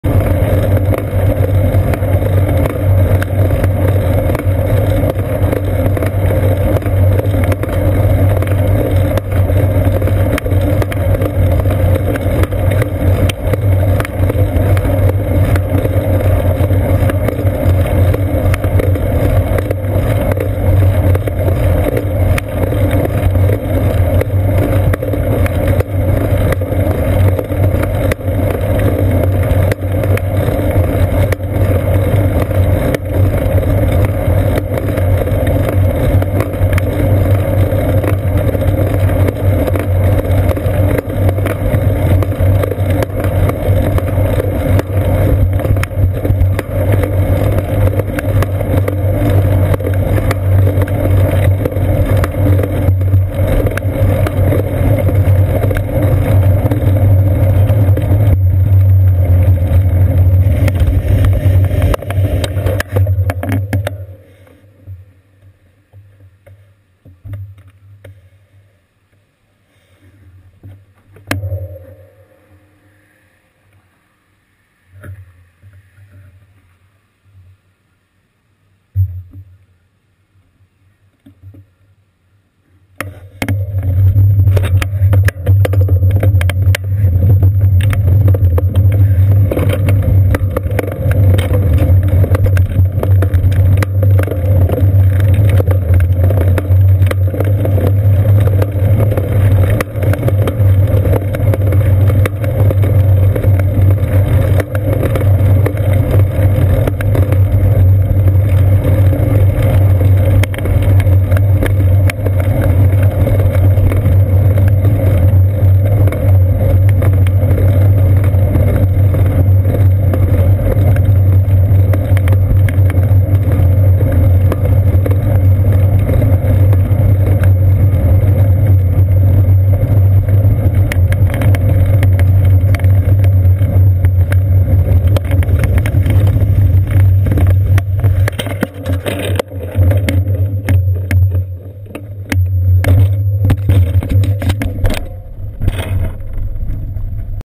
Florida Bike commute Thursday morning - Humid again - rear camera, part 1/2
The Florida late summer humidity is here. or back or something. it's 84% this morning making it a sweaty time biking through the streets of Safety Harbor and Clearwater, Florida on the way to work on the morning of August 13, 2015.